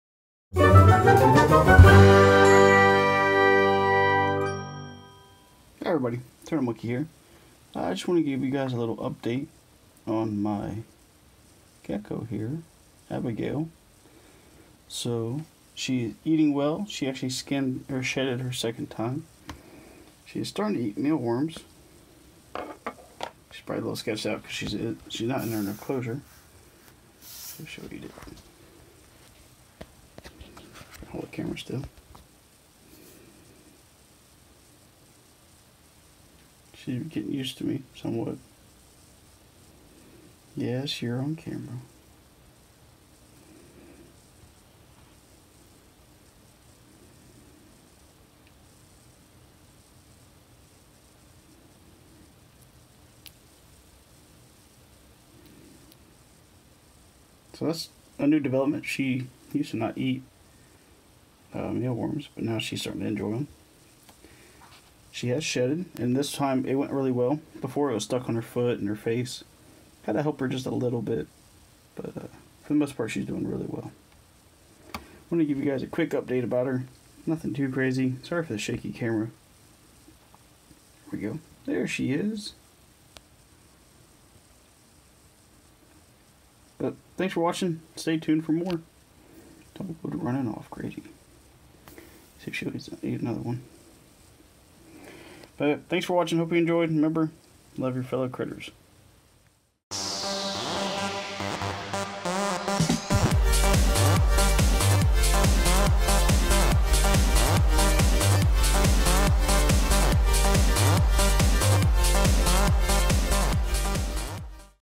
Mealworm Destoryer? - Update # 2
Here is a quick update about Abigail our Leopard Gecko. She is doing really well and is growing quickly. Thank you for tuning in and there will be more videos soon.

Links to good stuff.

Peat Moss
Frog Moss
Cork Bark
Wash Bottle